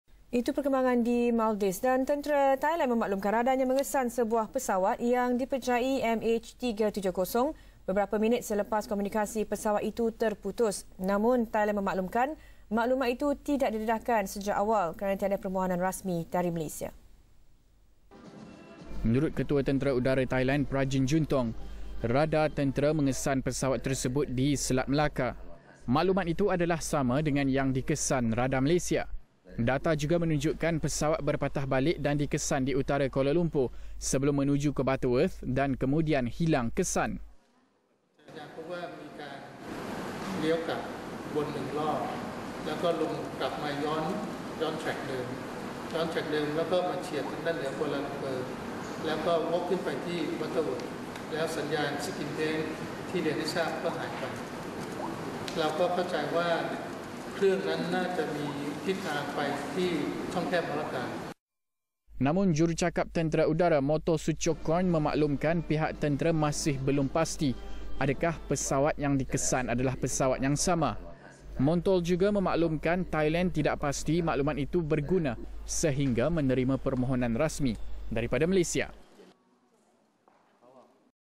Tentera Thailand kesan pesawat MH370 dalam radar
Tentera Thailand memaklumkan radarnya mengesan sebuah pesawat yang dipercayai MH370 beberapa minit selepas komunikasi pesawat itu terputus.

Namun memaklumkan maklumat itu tidak didedahkan sejak awal, kerana tiada permohonan rasmi dari Malaysia.